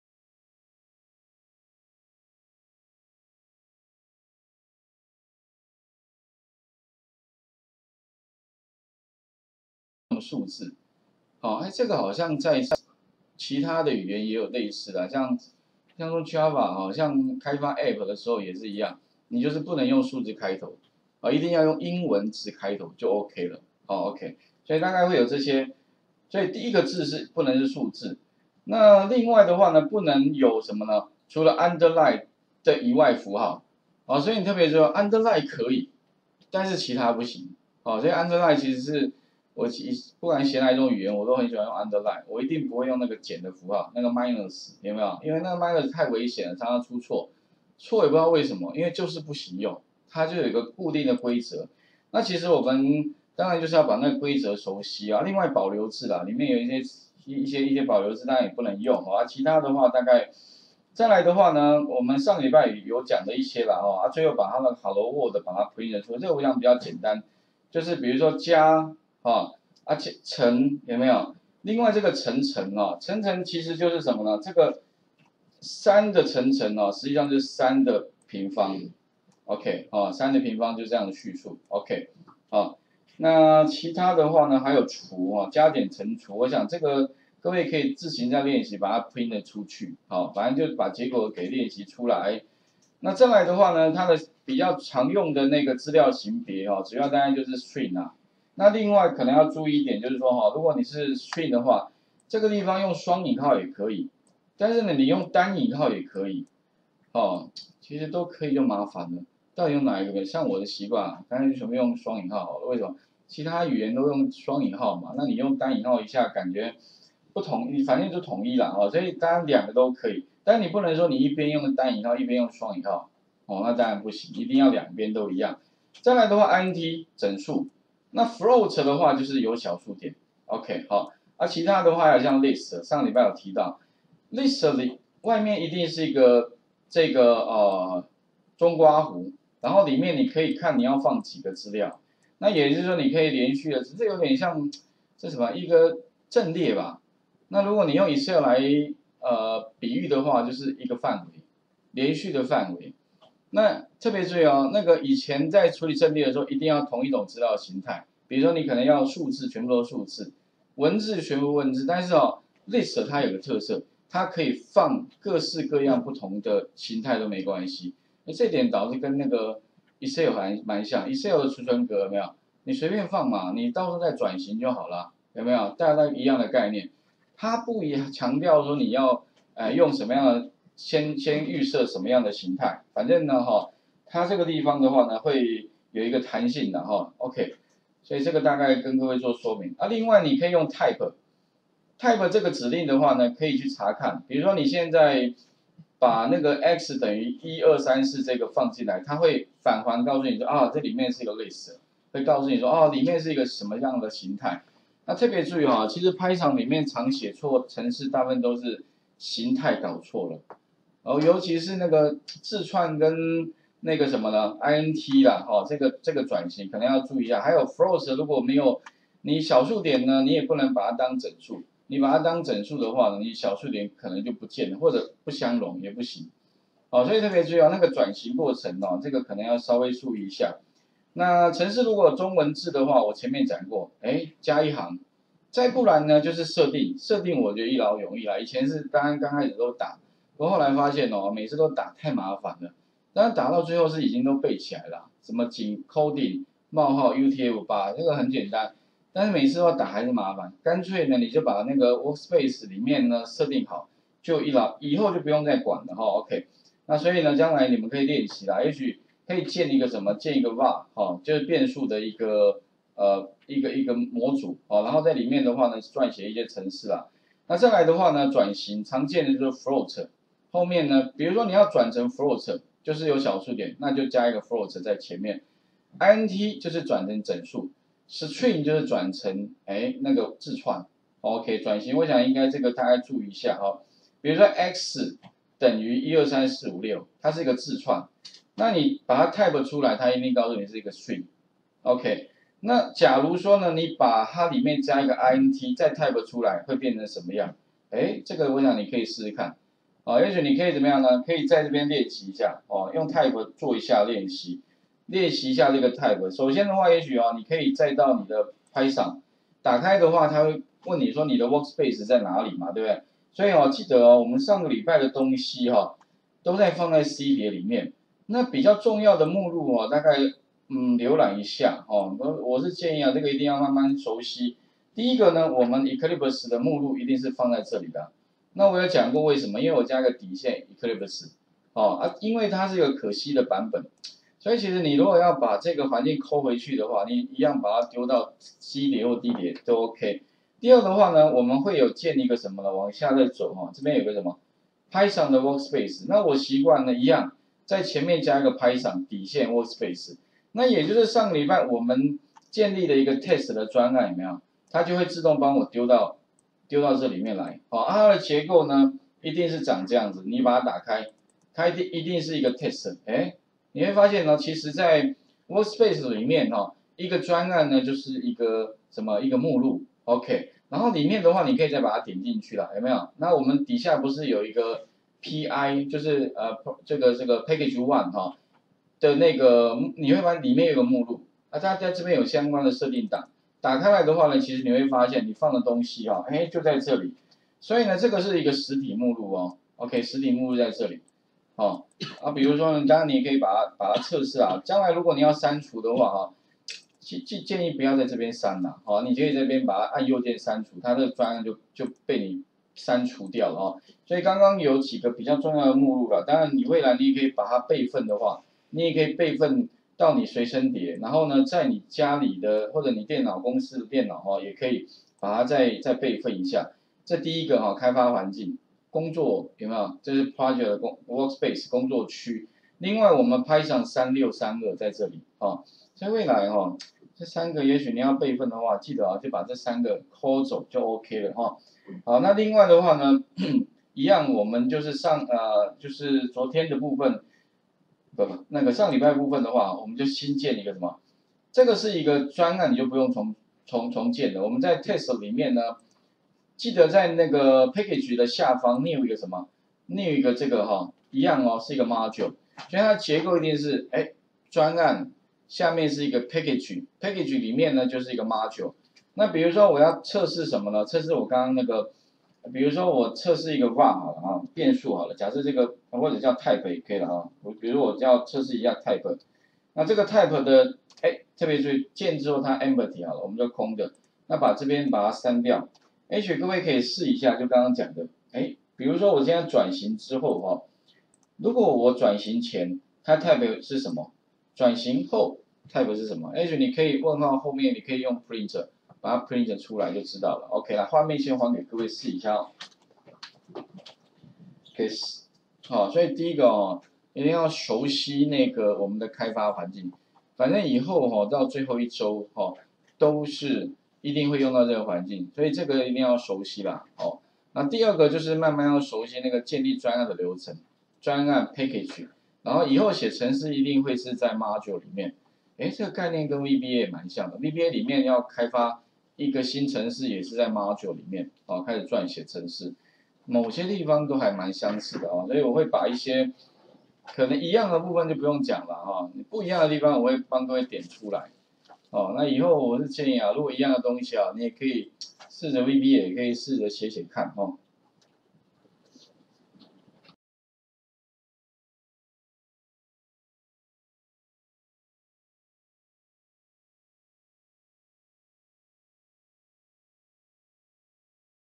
01 基本語法
從EXCEL VBA自動化到Python程式開發入門第2次上課(基本語法&用Type輸出資料型態&運算子與IF邏輯判斷&多重判斷與BMI練習題&其他練習題&成績計算與使用List型別&引用Math的PI與格式化資料&引用Random與產生亂數)

01_上周重點回顧與基本語法
02_用Type輸出資料型態
03_運算子與IF邏輯判斷
04_IF邏輯多重判斷與BMI練習題
05_IF邏輯多重判斷其他練習題
06_成績計算與使用List型別
07_引用Math的PI與格式化資料
08_引用Random與產生亂數

檔案處理
資料庫處理

上課用書：
參考書目

作者：葉難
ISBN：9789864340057
出版社：博碩文化
出版日期：2015/04/02

吳老師  106/9/9